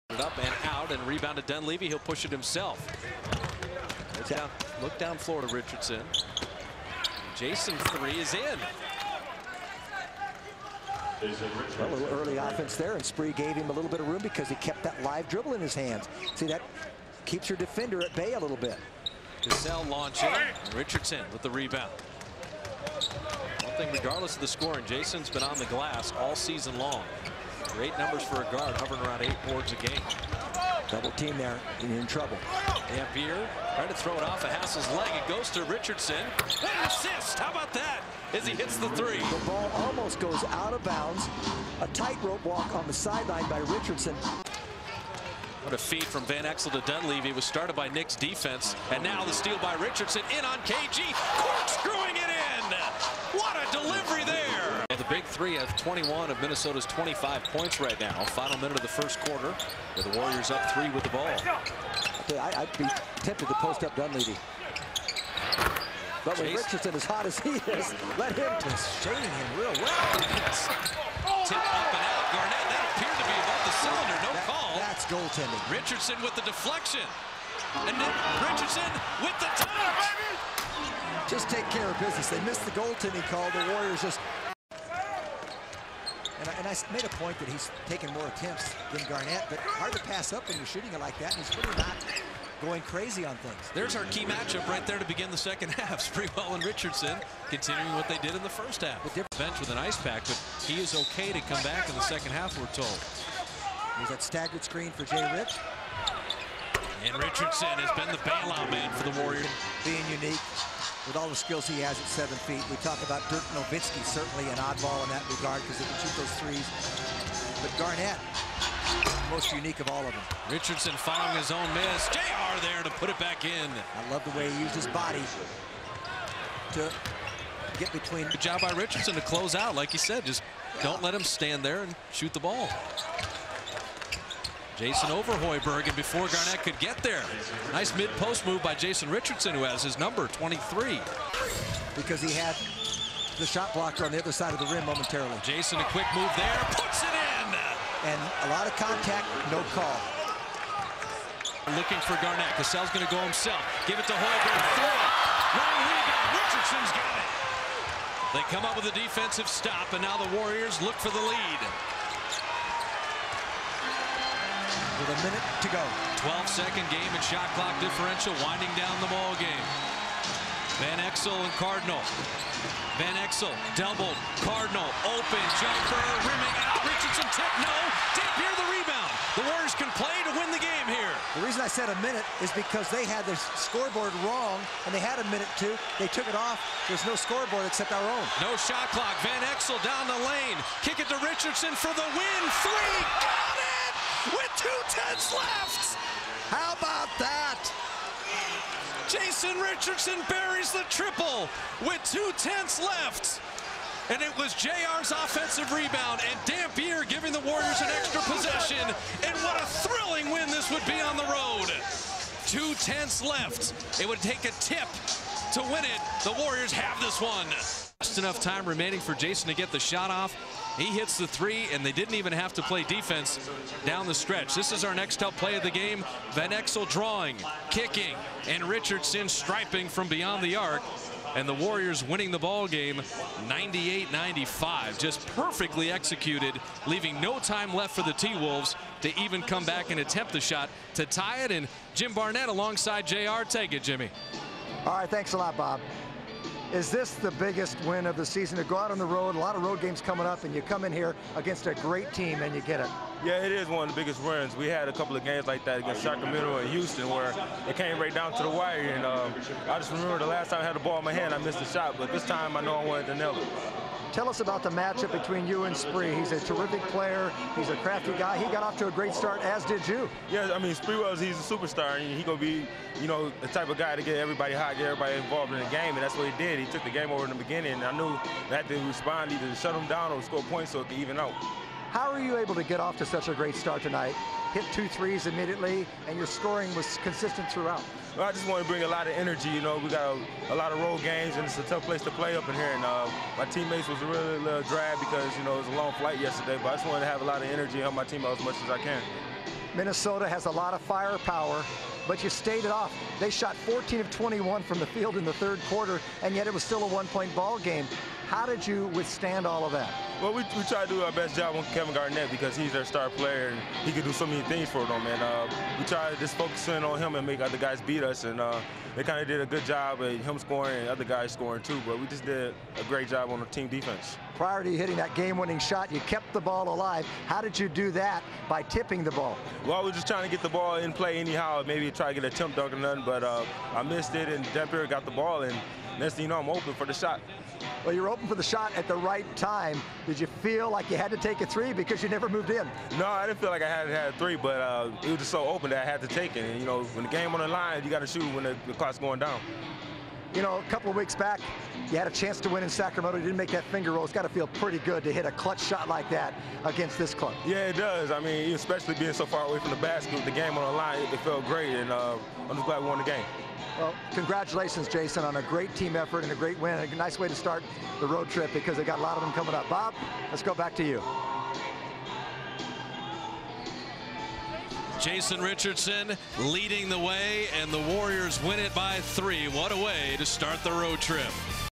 Jason Richardson - Warriors at Wolves - 12/9/03 (GAME-WINNER)
J-Rich` Highlights | Golden State Warriors @ Minnesota Timberwolves | Dec 9, 2003
(Game-winning shot!)